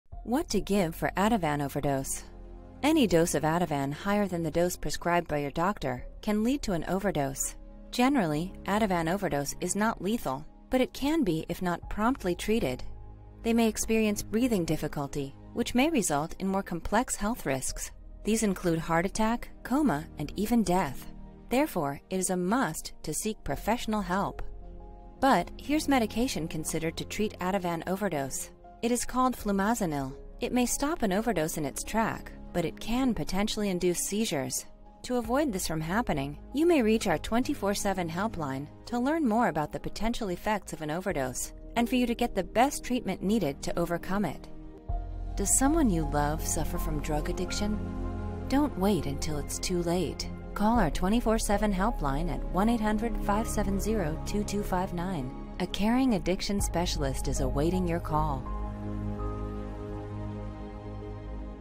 What To Give For Ativan Overdose?
Calling 911 is the fastest way to get treatment for an overdose and reduce the risk of long-term negative effects or death. If you are with someone who is overdosing, there are several actions you can take while waiting for emergency services, such as staying calm.